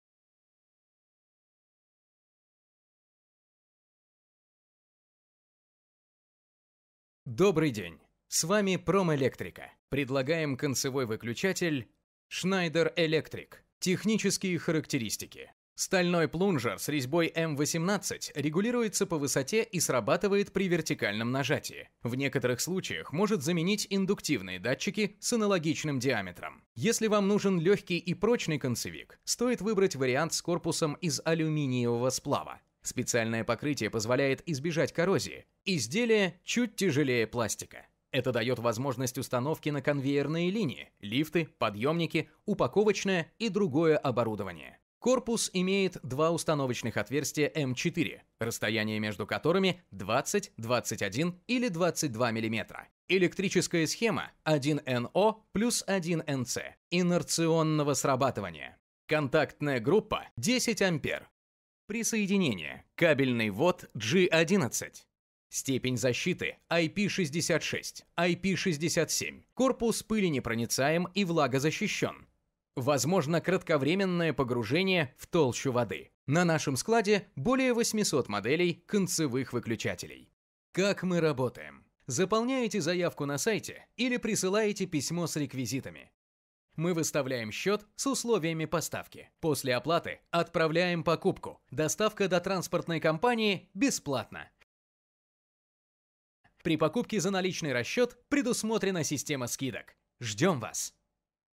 XCKD25H0G11 Концевой выключатель XCK D25H0G11
Предлагаем концевой выключатель Schneider Electric

Технические характеристики:
Стальной плунжер с резьбой M18 регулируется по высоте и срабатывает при вертикальном нажатии. В некоторых случаях может заменить индуктивные датчики с аналогичным диаметром.
Если Вам нужен легкий и прочный концевик, стоит выбрать вариант с корпусом из алюминиевого сплава. Специальное покрытие позволяет избежать коррозии. Изделие чуть тяжелее пластика. Это дает возможность установки на конвейерные линии, лифты, подъемники, упаковочное и другое оборудование.Корпус имеет 2 установочных отверстия М4, расстояние между которыми - 20, 21 или 22 мм.

Электрическая схема 1NO + 1NC Инерционного срабатывания
Контактная группа 10 ампер
Присоединение: кабельный ввод G-11
Степень защиты IP66, IP67

На нашем складе самый широкий ассортимент концевых выключателей в России, более 800 моделей.